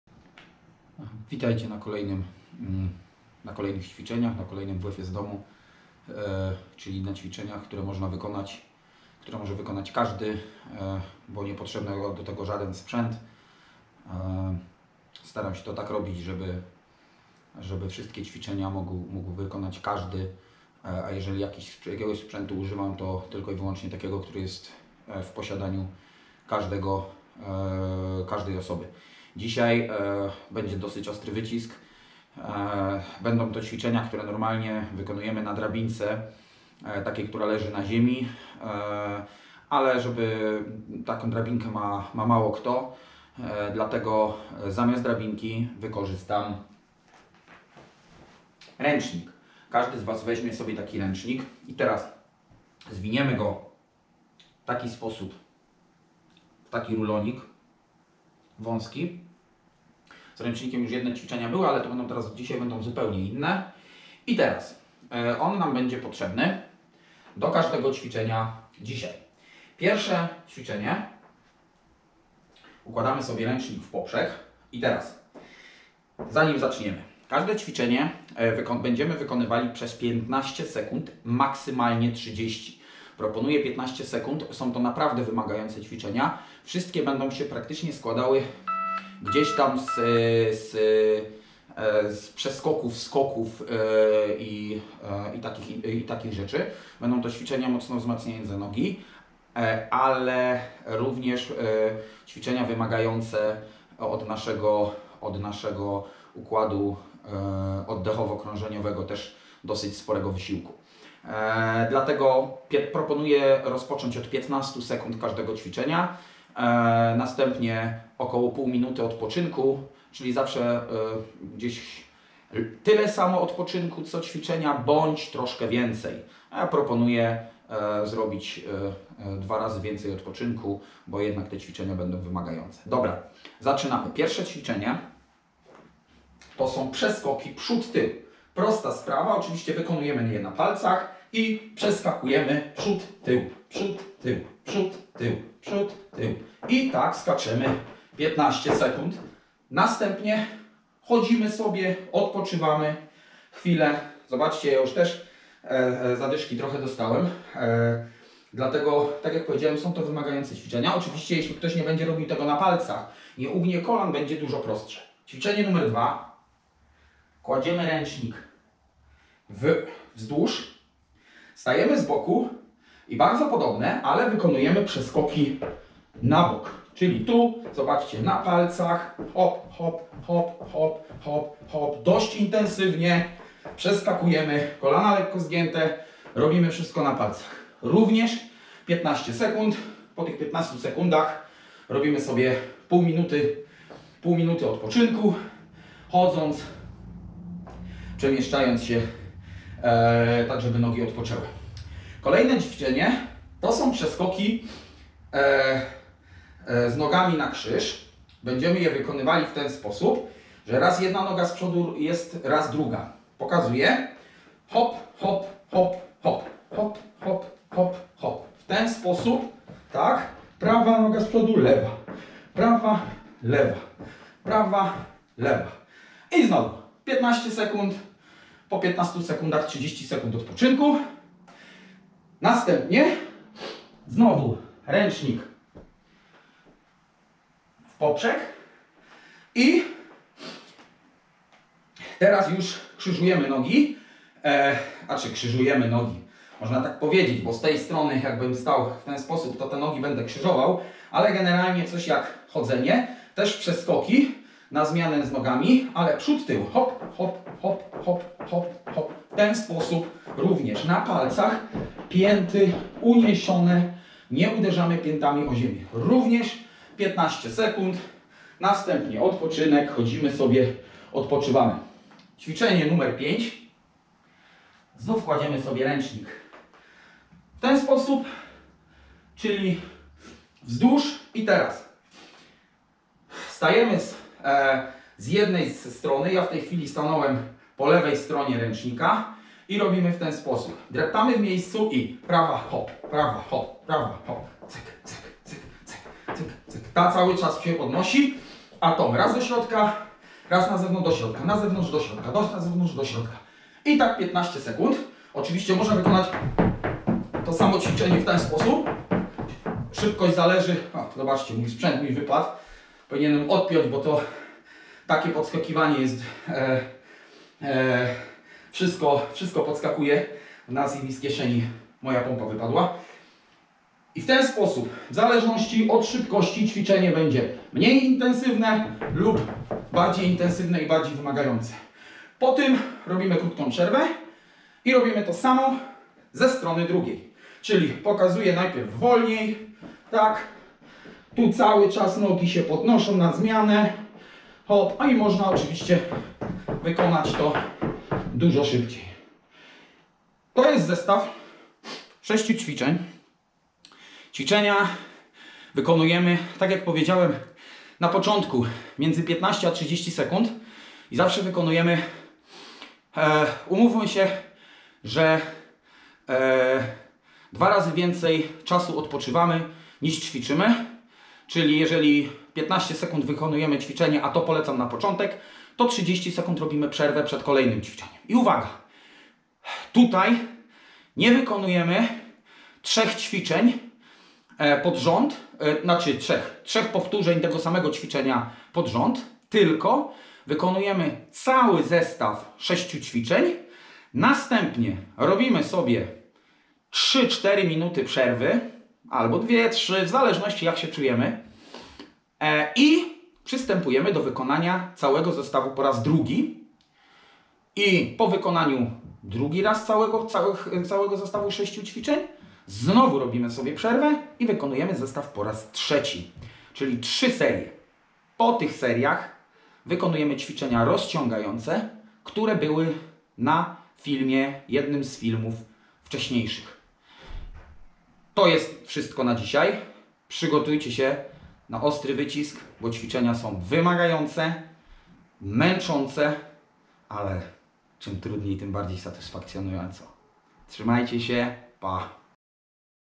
WF z domu Cz14, dynamika i szybkość
Ćwiczenia podobne do tych, które normalnie wykonujemy na drabince leżącej na ziemi